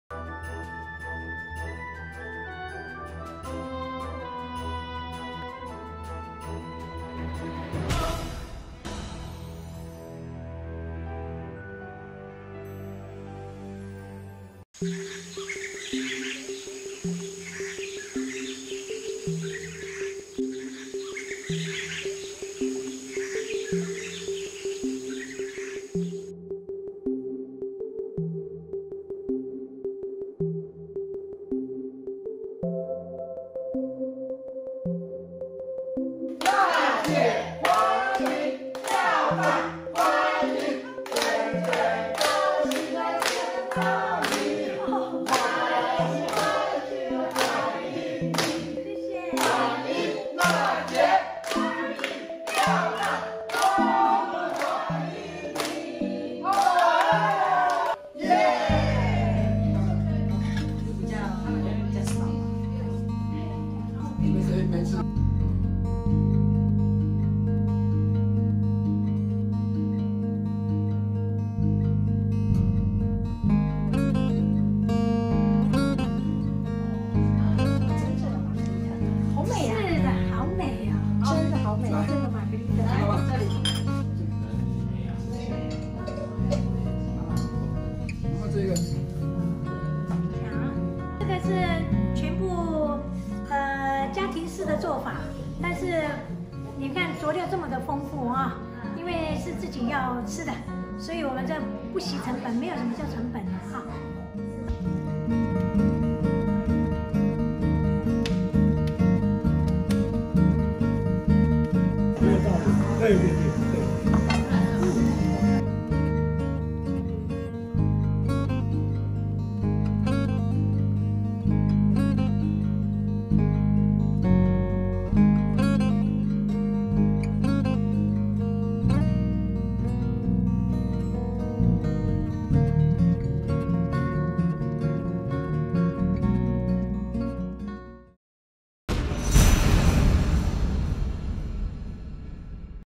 松下香蕉大姐爐烤素食Pizza教學
日本第一位外籍護國遍路先達,多年巡走遍路寺院推廣素食餐飲文化.2022不畏7＋7＋7檢疫,特地回台教授日本精進料理,分享異國寺院文化.本次大年初四作客雲起,熱身親授爐烤素食Pizza,果然不同凡響！